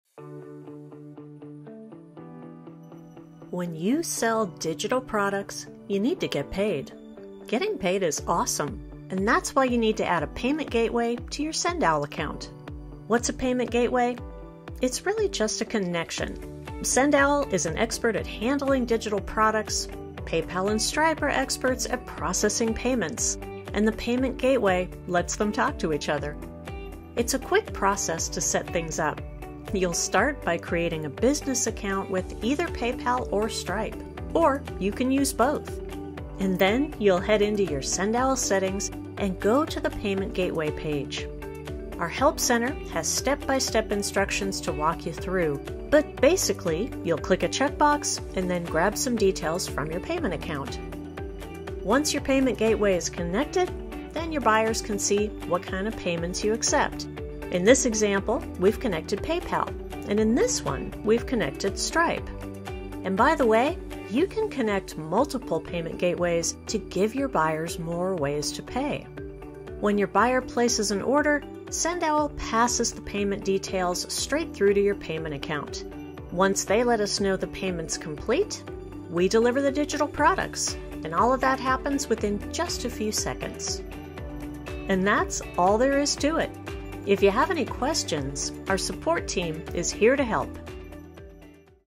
SendOwl: Why do I need a payment gateway?
Sell digital products anywhere you go online, with SendOwl.

In this video, we're learning about one quick setting in your SendOwl account that lets you get paid: connecting a payment gateway.